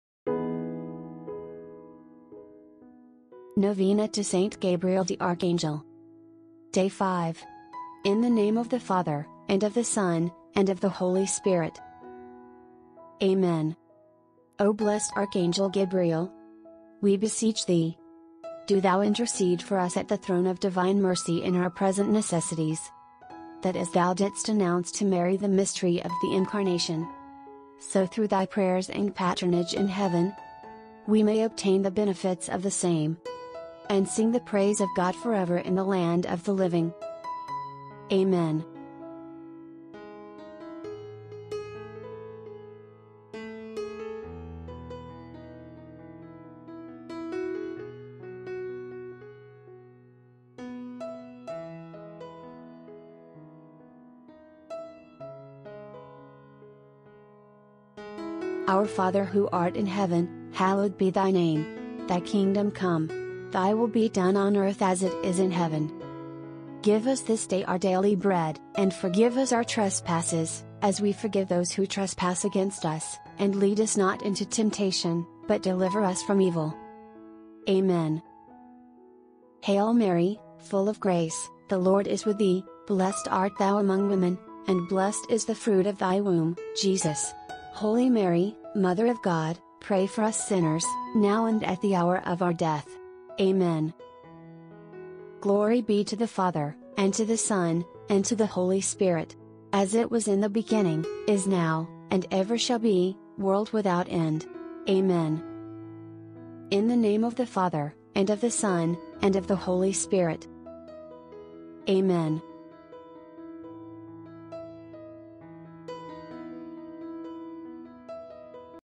NOVENA TO ST GABRIEL THE ARCHANGEL | DAY 5
Novena Begins: September 20
Feast Day: September 29
Patron Saint of messengers, Postal workers, telecommunication workers
O blessed Archangel Gabriel, we beseech thee, do thou intercede for us at the throne of divine mercy in our present necessities, that as thou didst announce to Mary the mystery of the Incarnation, so through thy prayers and patronage in heaven we may obtain the benefits of the same, and sing the praise of God forever in the land of the living. Amen
Say 1: Our Father…
Say 1: Hail Mary…
Say 1: Glory Be…